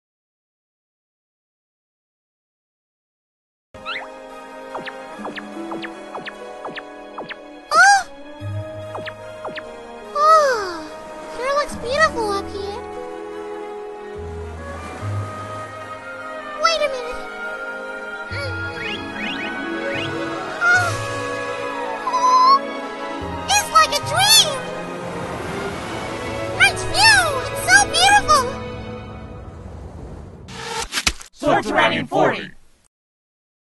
IF POKÉMON TALKED: Eevee Observes the Sea from the Ship's Perspective
Here's the last part from the If Pokémon Talked Where Are You Going Eevee (Episode 1) video that couldn't fully make it on YouTube. In this part Eevee ends up on a ship, it looks up at the front, and notices the beautiful view from the ship.

Full "Where Are You Going Eevee" If Pokémon Talked video on Twitter

Pokémon voiced in the video:
Eevee. Wingull

Episode: "Where Are You Going Eevee? (Part 1)"/"Liller and the Staff"